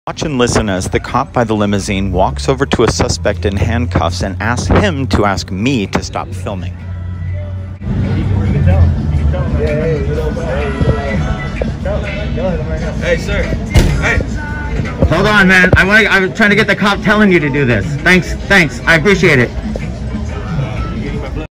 A cop tells a suspect in handcuffs to ask me to stop filming. This is a civil rights violation!
This is a huge civil rights violation. This man is in handcuffs and is being ordered by a police officer to ask me to stop filming. I hope he gets fired. This is the LAPD in Hollywood On the night of October 7, 2020.